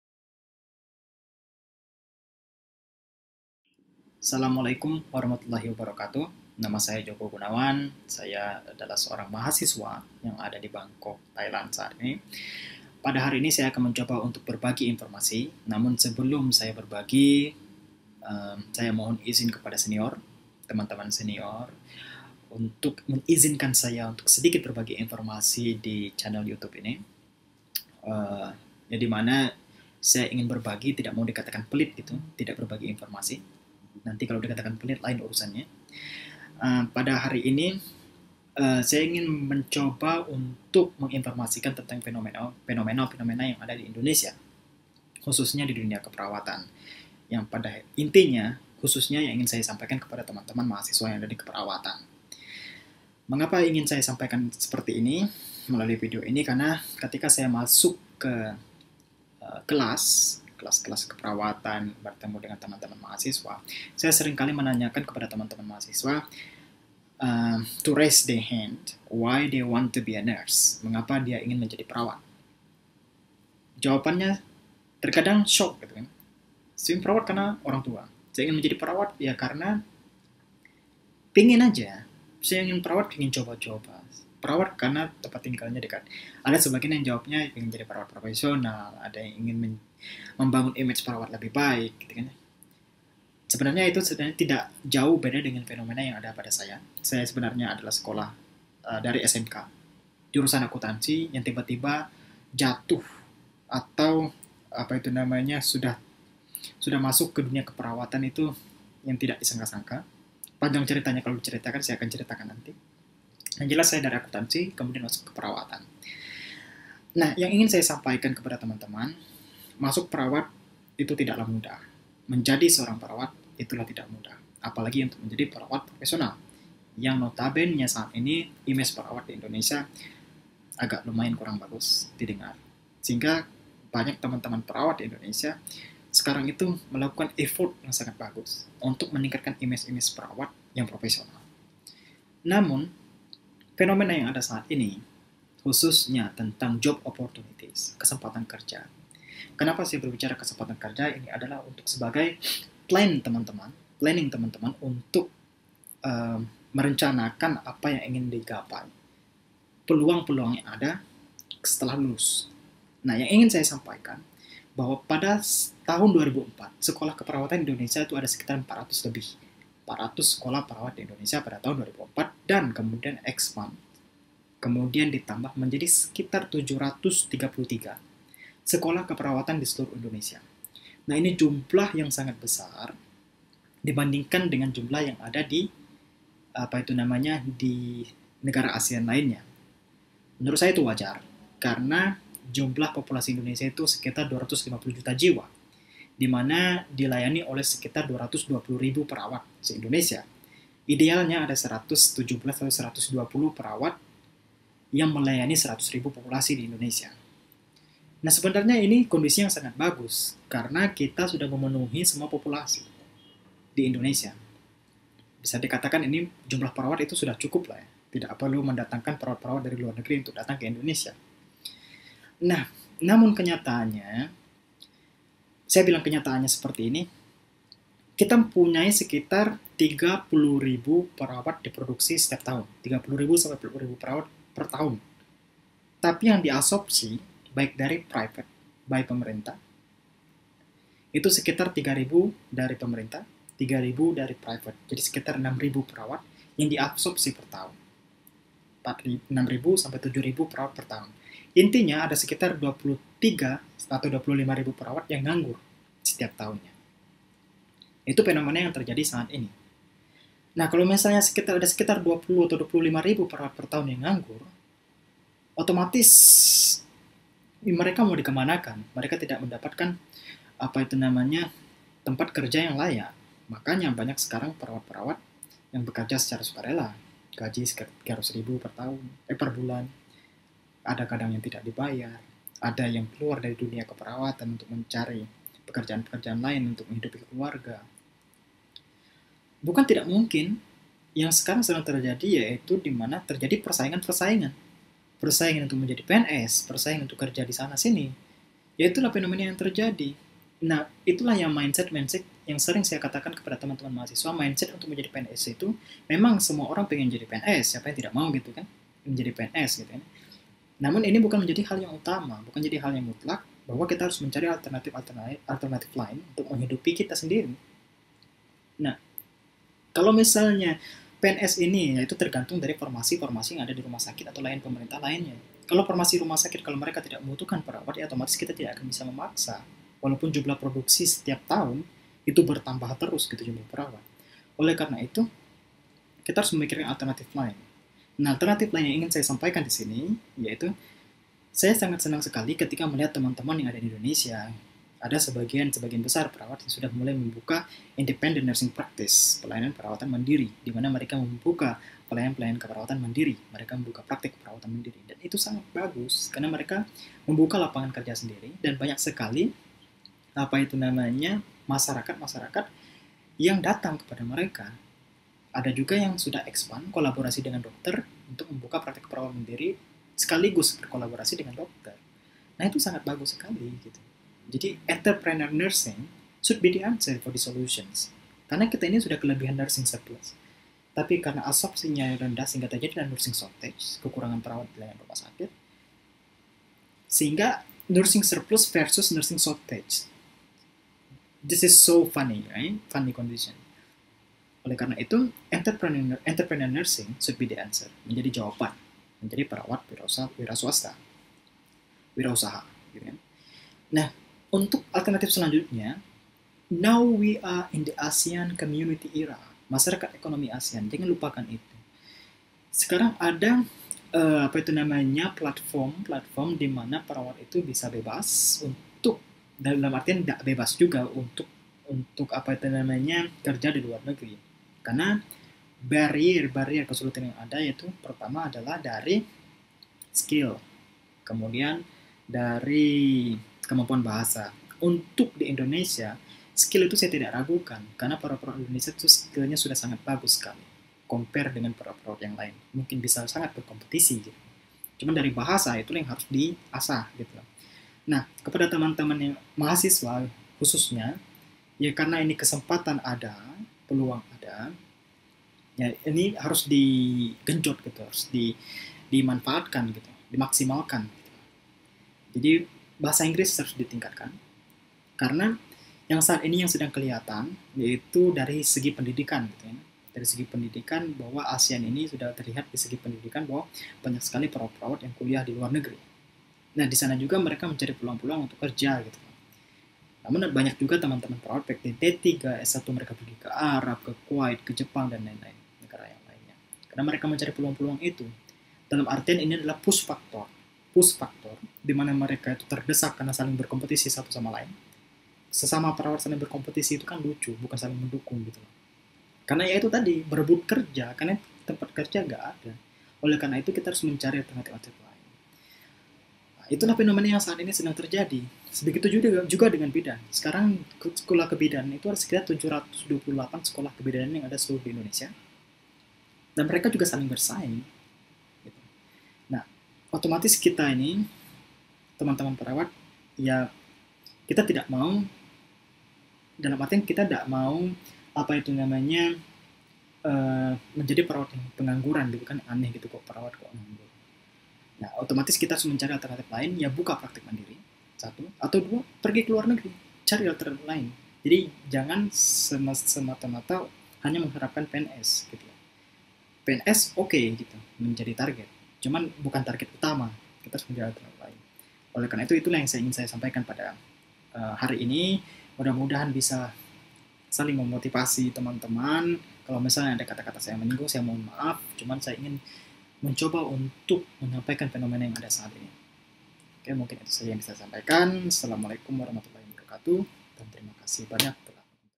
Ingin menjadi perawat? Pikirkan Kembali!! - (Pastikan nonton video sampai habis baru komen)
Video ini ditujukan kepada mahasiswa-mahasiswa keperawatan tentang gambaran fenomena keperawatan di Indonesia. Semoga bermanfaat !